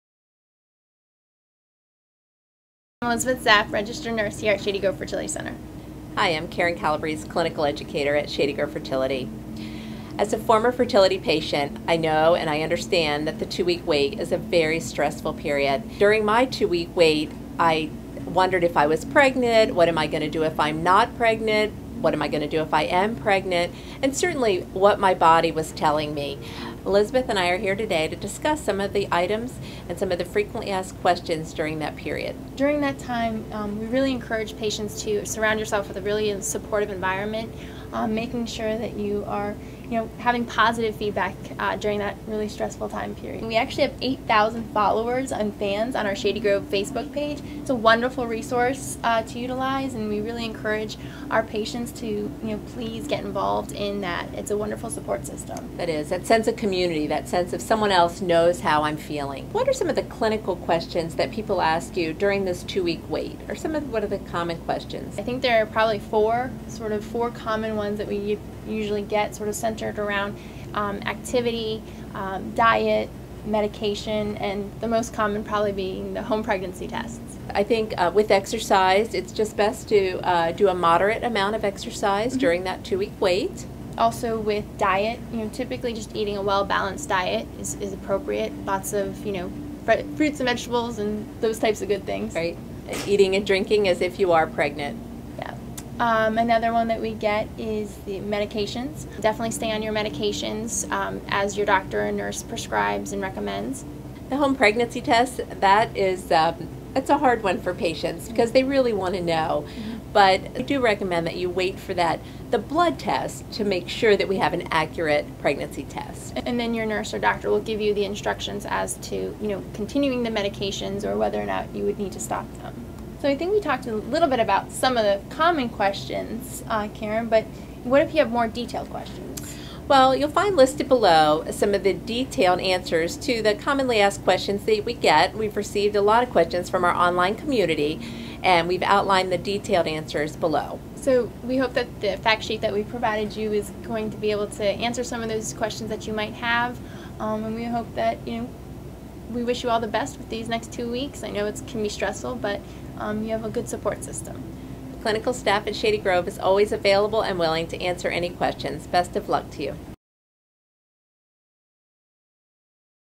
Two Week Wait | Fertility Nurse's Tips
You're not alone -- every patient has to endure the same two week time frame between the end of their treatment and when they can take a pregnancy test.  During that time, you might be experiencing different symptoms and questions that you had not encountered in the past.

Shady Grove Fertility Center's nurses are trained to answer your questions. We sat down with nurses Karen Calabrese, RN, and Elizabeth Zapp, RN, to ask them about some of the most common questions they are asked from patients during their two week wait.